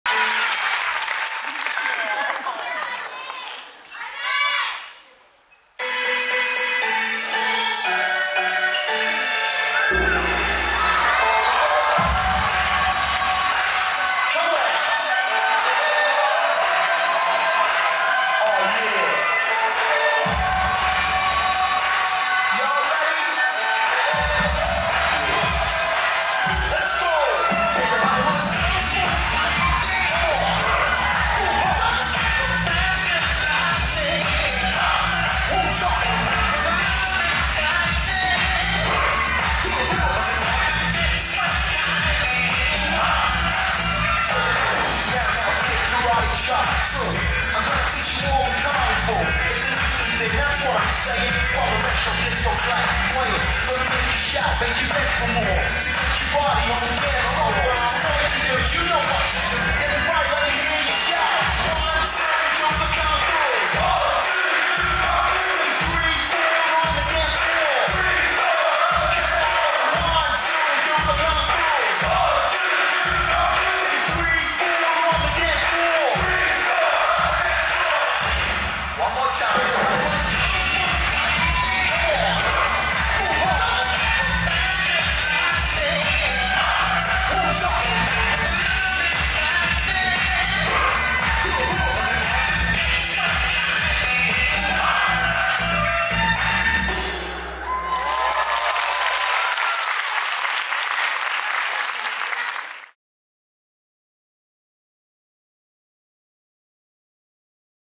김희원_초등부 솔로_female star junior vol.5
김희원_초등부 솔로
female star junior vol.5
초.중.고 걸스 크루 & 솔로 댄스 컨테스트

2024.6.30
부천 복사골 아트홀

*judge
-FUNKY Y
-LOCKER ZEE
-HAEJUN
-ONNY
-YEJUN
-TORCH

*mc
-KYAM

*dj
-JOOPOP

*ghestshow
-ALLEYOOP
-WELLBOSS CREW
-SHO CREW
-SI_A
-DANA
-박시연

design by
-YOUNG

video by
-YEN.FILM

host
-DANCER'S NIGHT(HANS)